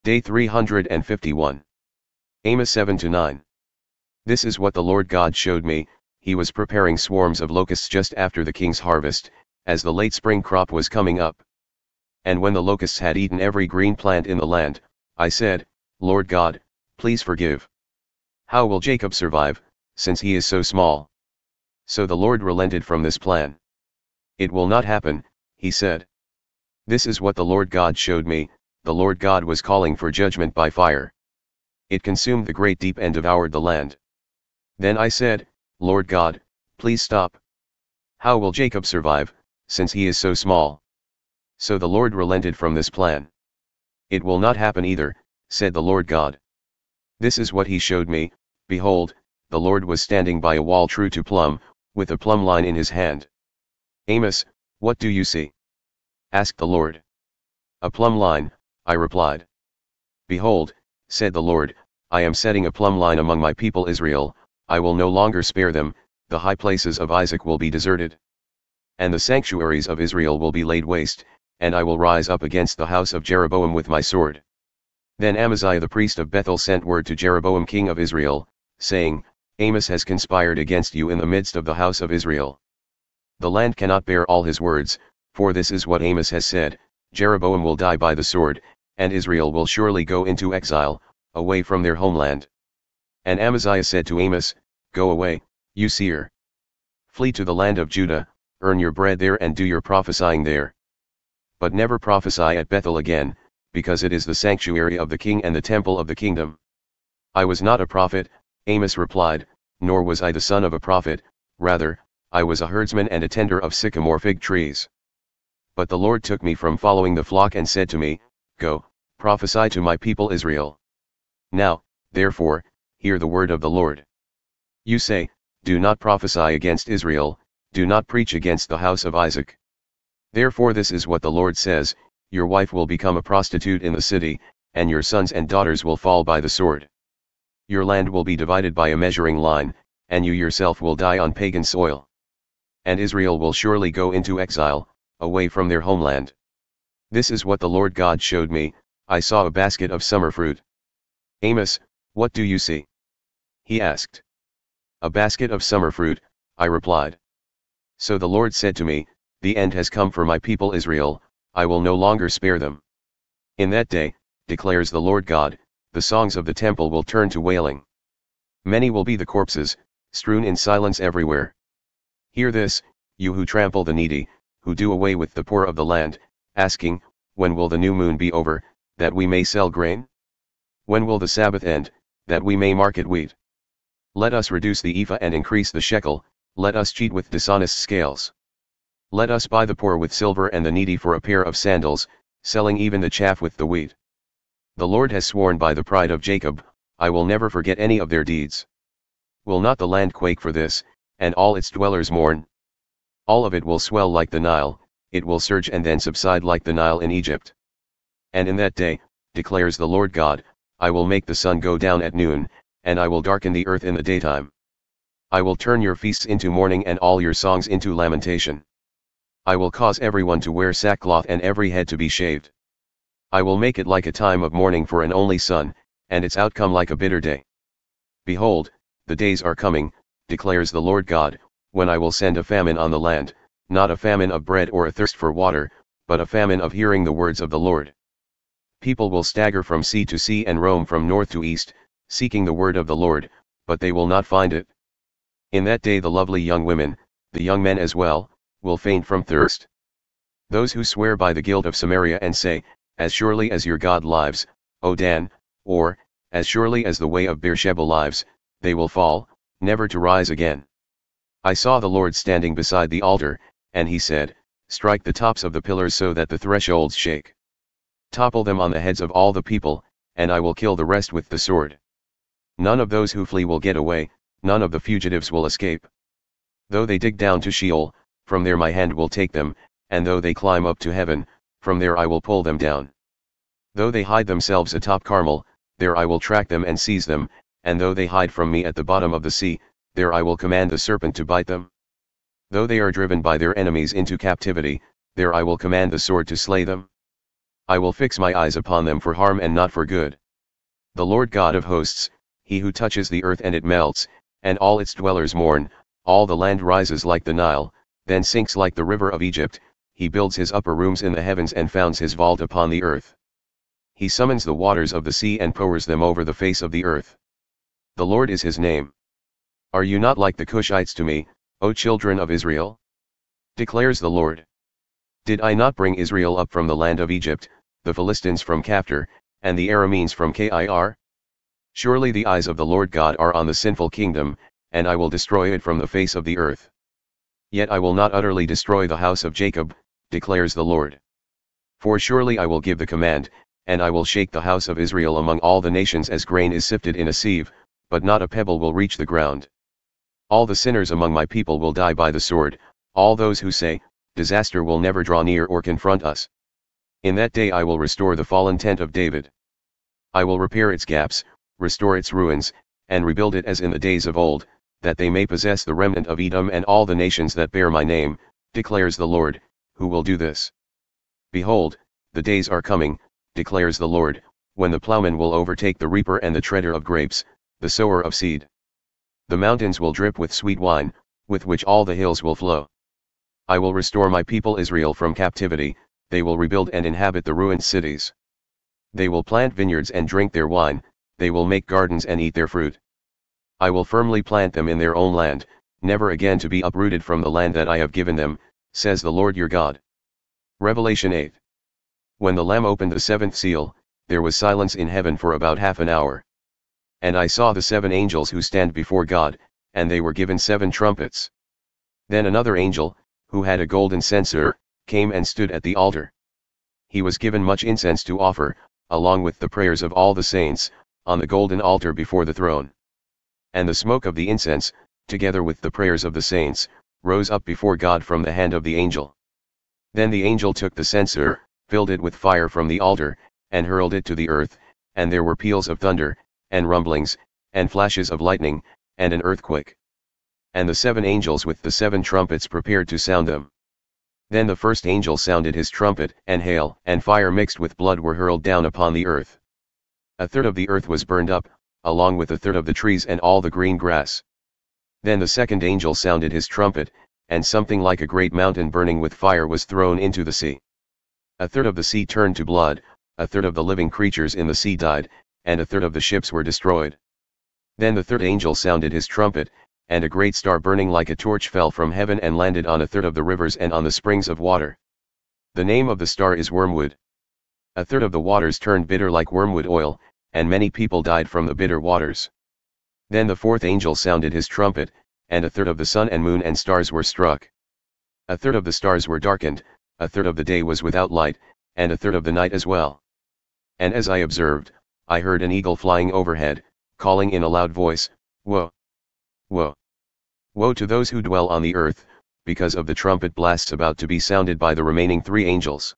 Day 351, Amos 7 - 9; Revelation 8
Day 351

Amos 7 to 9

This is what the Lord GOD showed me: He was preparing swarms of locusts just after the king’s harvest, as the late spring crop was coming up. And when the locusts had eaten every green plant in the land, I said, “Lord GOD, please forgive! How will Jacob survive, since he is so small?” So the LORD relented from this plan. “It will not happen,” He said. This is what the Lord GOD showed me: The Lord GOD was calling for judgment by fire. It consumed the great deep and devoured the land. Then I said, “Lord GOD, please stop! How will Jacob survive, since he is so small?” So the LORD relented from this plan. “It will not happen either,” said the Lord GOD. This is what He showed me: Behold, the Lord was standing by a wall true to plumb, with a plumb line in His hand. “Amos, what do you see?” asked the LORD. “A plumb line,” I replied. “Behold,” said the Lord, “I am setting a plumb line among My people Israel; I will no longer spare them: The high places of Isaac will be deserted, and the sanctuaries of Israel will be laid waste; and I will rise up against the house of Jeroboam with My sword.” Then Amaziah the priest of Bethel sent word to Jeroboam king of Israel, saying, “Amos has conspired against you in the midst of the house of Israel. The land cannot bear all his words, for this is what Amos has said: ‘Jeroboam will die by the sword, and Israel will surely go into exile, away from their homeland.’” And Amaziah said to Amos, “Go away, you seer! Flee to the land of Judah; earn your bread there and do your prophesying there. But never prophesy at Bethel again, because it is the sanctuary of the king and the temple of the kingdom.” “I was not a prophet,” Amos replied, “nor was I the son of a prophet; rather, I was a herdsman and a tender of sycamore-fig trees. But the LORD took me from following the flock and said to me, ‘Go, prophesy to My people Israel.’ Now, therefore, hear the word of the LORD. You say: ‘Do not prophesy against Israel; do not preach against the house of Isaac.’ Therefore this is what the LORD says: ‘Your wife will become a prostitute in the city, and your sons and daughters will fall by the sword. Your land will be divided by a measuring line, and you yourself will die on pagan soil. And Israel will surely go into exile, away from their homeland.’” This is what the Lord GOD showed me: I saw a basket of summer fruit. “Amos, what do you see?” He asked. “A basket of summer fruit,” I replied. So the LORD said to me, “The end has come for My people Israel; I will no longer spare them.” “In that day,” declares the Lord GOD, “the songs of the temple will turn to wailing. Many will be the corpses, strewn in silence everywhere!” Hear this, you who trample the needy, who do away with the poor of the land, asking, “When will the New Moon be over, that we may sell grain? When will the Sabbath end, that we may market wheat? Let us reduce the ephah and increase the shekel; let us cheat with dishonest scales. Let us buy the poor with silver and the needy for a pair of sandals, selling even the chaff with the wheat!” The LORD has sworn by the Pride of Jacob: “I will never forget any of their deeds. Will not the land quake for this, and all its dwellers mourn? All of it will swell like the Nile; it will surge and then subside like the Nile in Egypt. And in that day, declares the Lord GOD, I will make the sun go down at noon, and I will darken the earth in the daytime. I will turn your feasts into mourning and all your songs into lamentation. I will cause everyone to wear sackcloth and every head to be shaved. I will make it like a time of mourning for an only son, and its outcome like a bitter day. Behold, the days are coming, declares the Lord GOD, when I will send a famine on the land - not a famine of bread or a thirst for water, but a famine of hearing the words of the LORD. People will stagger from sea to sea and roam from north to east, seeking the word of the LORD, but they will not find it. In that day the lovely young women - the young men as well - will faint from thirst. Those who swear by the guilt of Samaria and say, ‘As surely as your god lives, O Dan,’ or, ‘As surely as the way of Beersheba lives’ - they will fall, never to rise again.” I saw the Lord standing beside the altar, and He said: “Strike the tops of the pillars so that the thresholds shake. Topple them on the heads of all the people, and I will kill the rest with the sword. None of those who flee will get away; none of the fugitives will escape. Though they dig down to Sheol, from there My hand will take them; and though they climb up to heaven, from there I will pull them down. Though they hide themselves atop Carmel, there I will track them and seize them; and though they hide from Me at the bottom of the sea, there I will command the serpent to bite them. Though they are driven by their enemies into captivity, there I will command the sword to slay them. I will fix My eyes upon them for harm and not for good.”